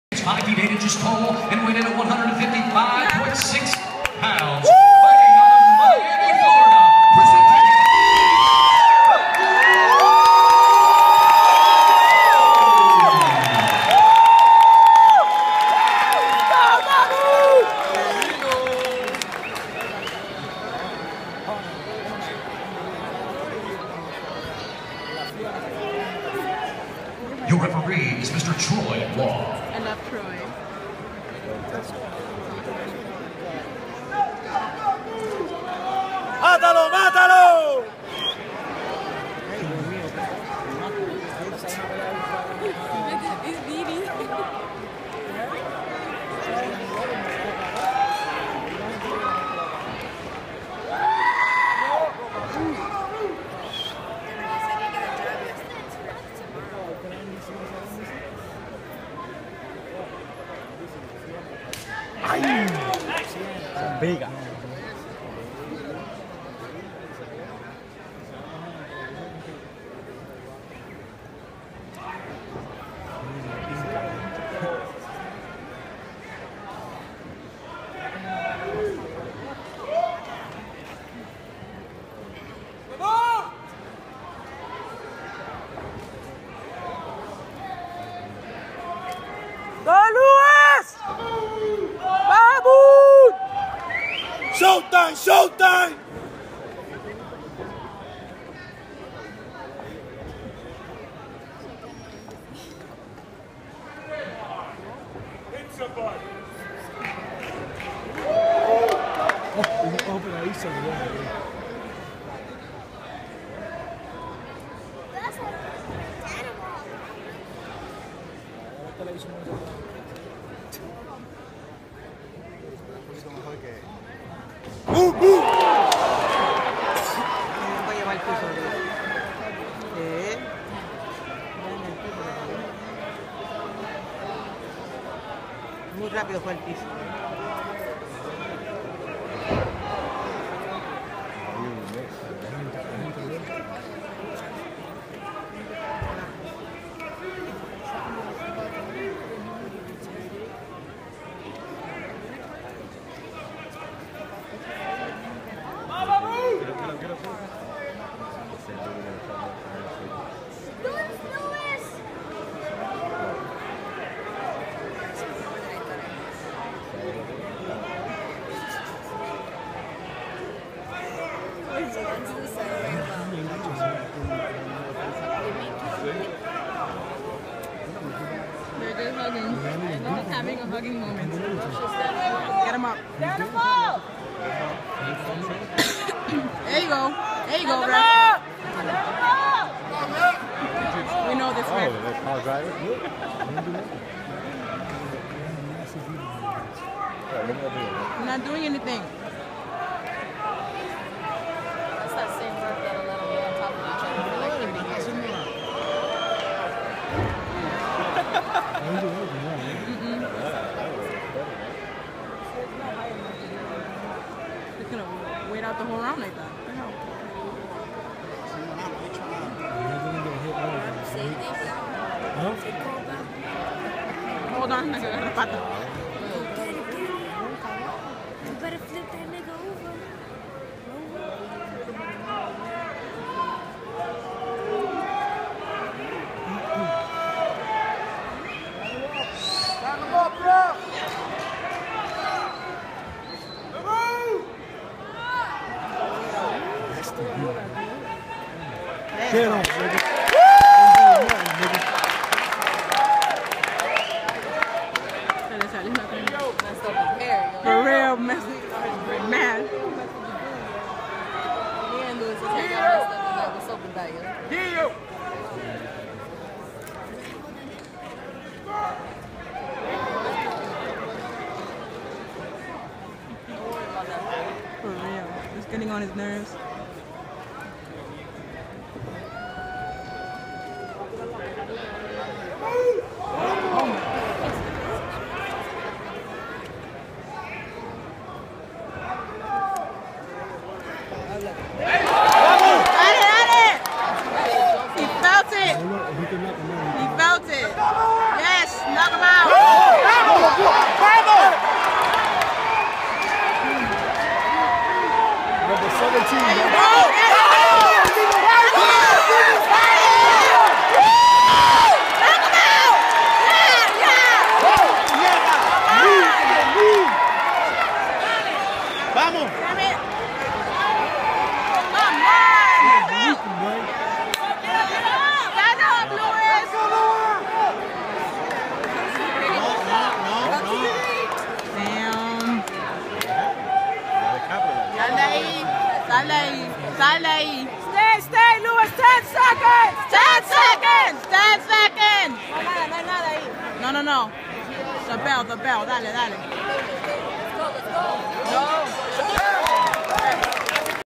Luis Baboon Palomino vs Jorge Macaco Patino round 1
WSOF 8 at the Hard Rock Cafe and Casino, Hollywood Fl